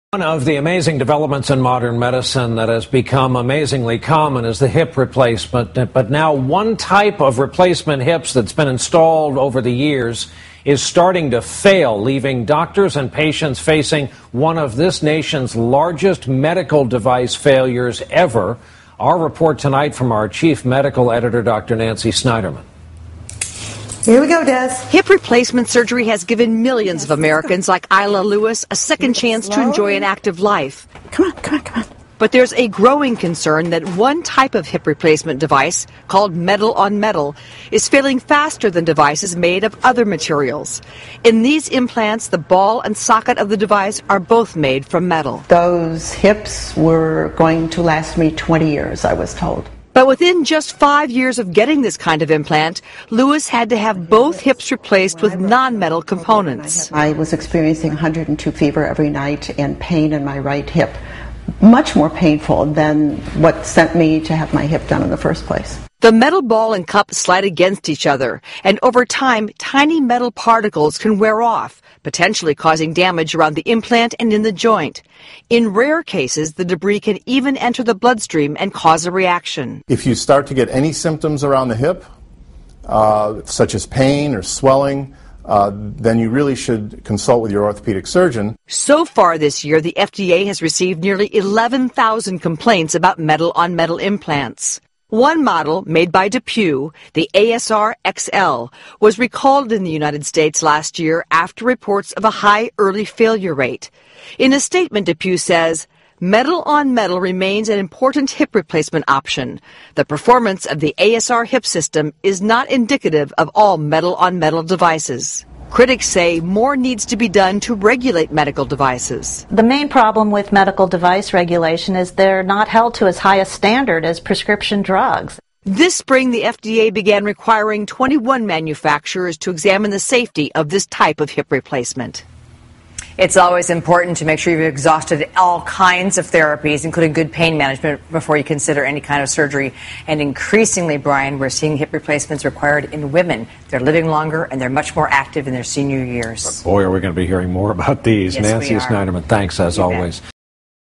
Metal On Metal Implants- MOM Hip Implant Failures On The Rise
Metal on metal hip implants such as the Depuy ASR hip resurfacing system, are failing at an alarming rate. As the implants wear, metals such as chromium and cobalt are released into the body at toxic levels resulting in metal toxicity ( metallosis), inflammation and permanant tissue damage. It can also enter the bloodstream, raising concerns about deafness, dementia and heart failure.

If you or a loved one has received a Depuy ASR metal on metal hip implant, please contact Hummer Law Offices for a free consultation at or visit us on the web